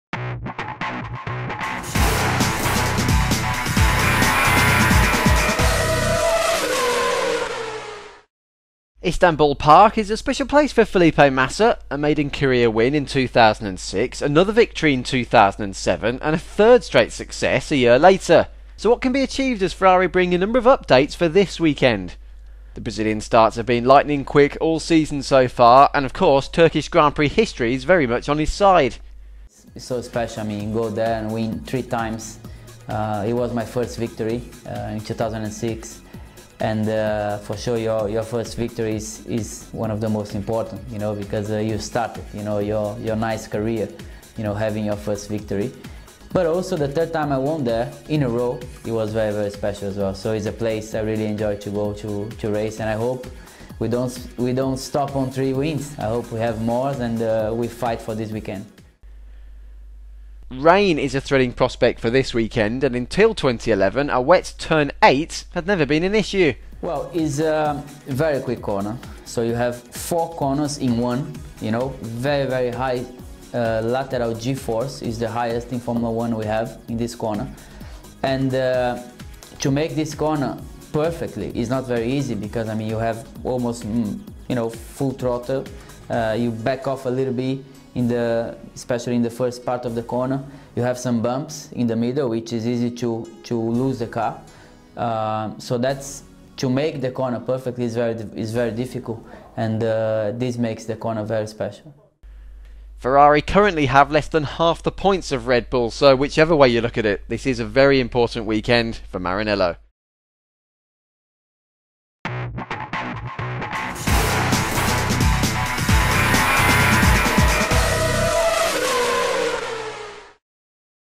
Massa looks on to Istanbul
Istanbul Park has traditionally been a happy hunting ground for Felipe Massa, even if the last two years have suggested otherwise. The Ferrari looks ahead to this weekend's 2011 Turkish Grand Prix.